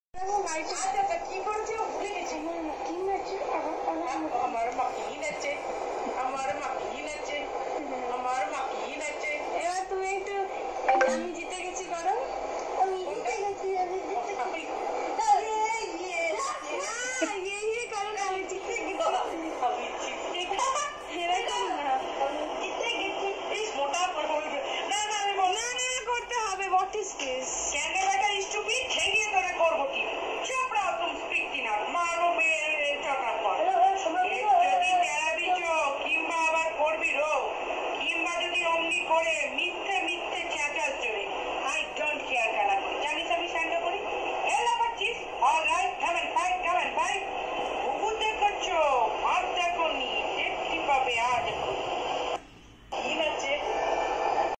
Rhymes to the Children
Mother Srabani Pathak
Paulami and brother Kanishka Pathak listening to Abol Tabol of Satyajit Ray
in a lighter mood
pic.   2006 approx